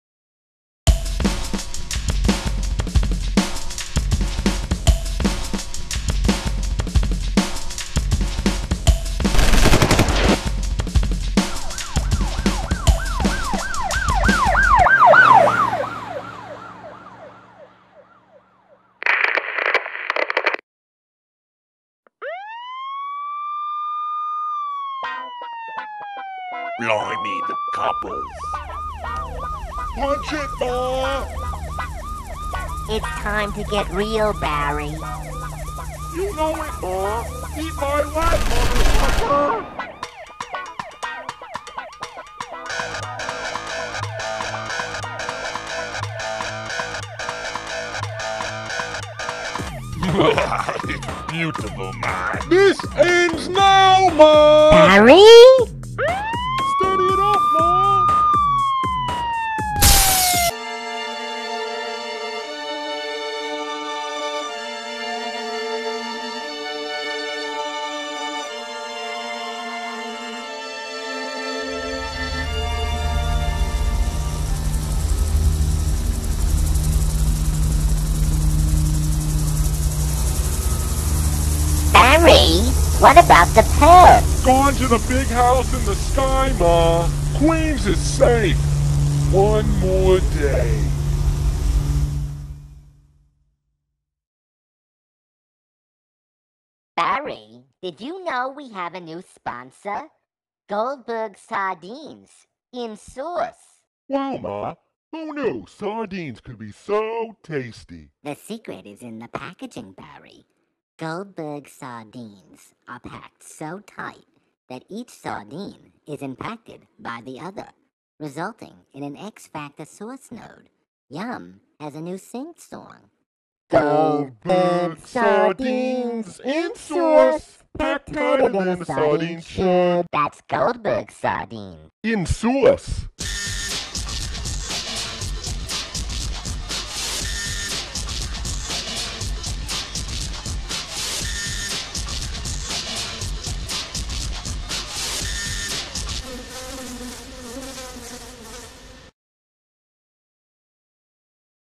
Funny Comedy Short - My ma and me are cops: hot mamma
A madman is on the loose and Barry and Ma aren't going to let him get away.

I made big bucks making short form films.